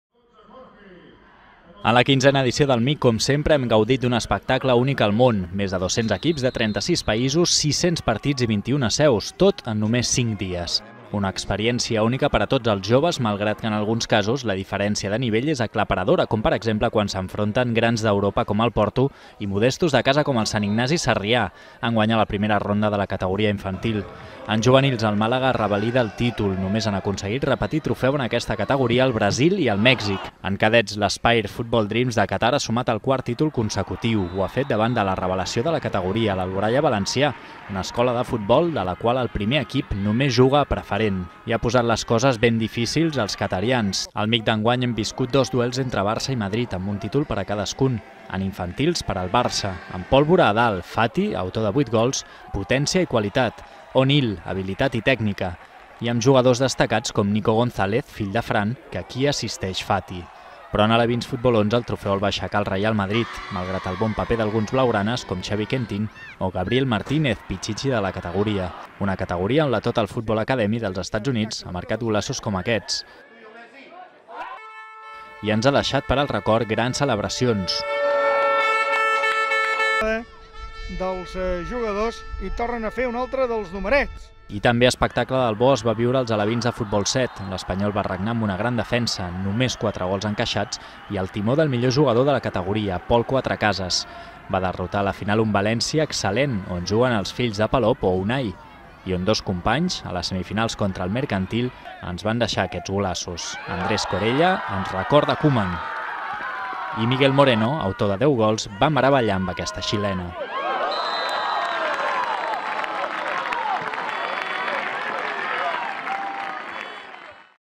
La 15a edició del MIC reuneix més de 200 equips de 36 països a la Costa Brava durant cinc dies
L’equip cadet de la Unió Esportiva Olot ha arribat als vuitens de final del torneig de futbol base MIC de la Costa Brava. El cadet de l’Olot és el que més ha escalat en aquest torneig. L’equip de l’Aspire va proclamar-se campió del MIC cadet, el Màlaga va guanyar en la categoria juvenil, el Barça es va fer amb el títol infantil, el Reial Madrid amb l’aleví de Futbol 11 i l’Espanyol ho va fer en la categoria aleví de Futbol 7. Dos equips del Club Atlètic Garrotxa van participar en el MIC Integra, que va tenir com a guanyador al MAP Abadessenc.